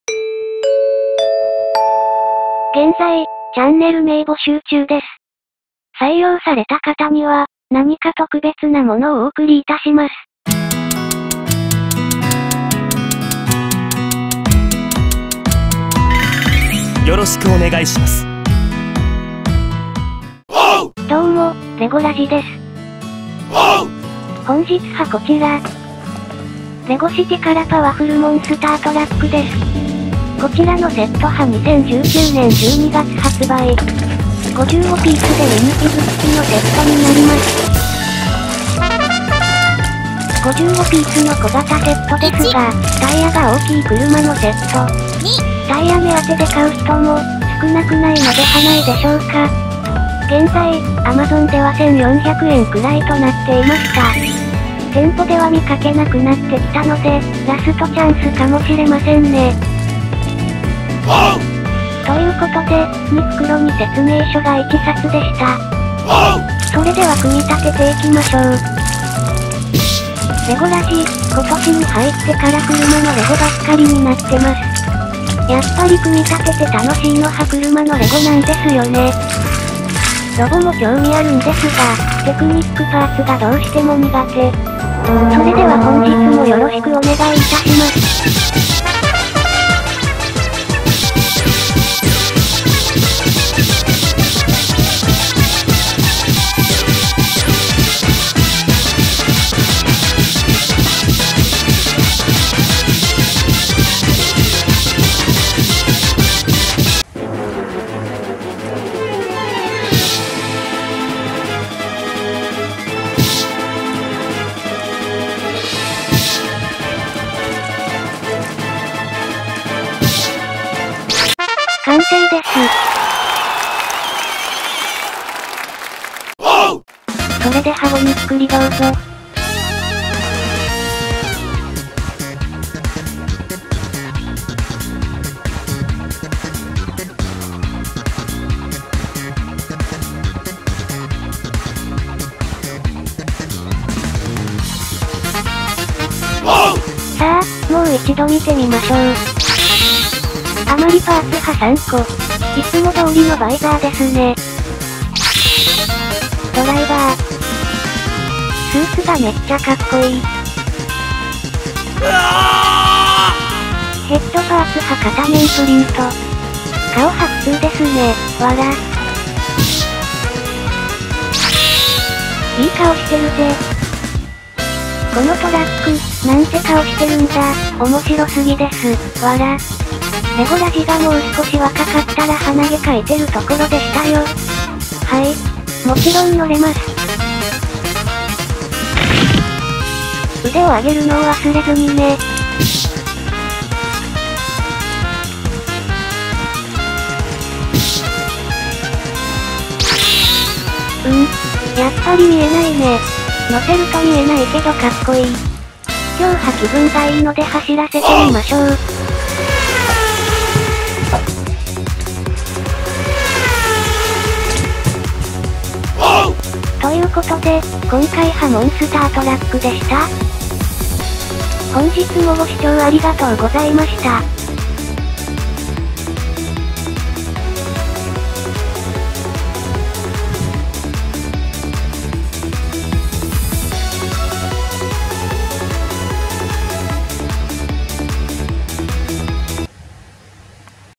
レゴシティ パワフル モンスタートラック LEGO CITY 60251 Monster Truck
ミニフィグ付き。大きいタイヤのバギーです。
やっぱり車のレゴは楽しい。

♦Amazonで価格をチェック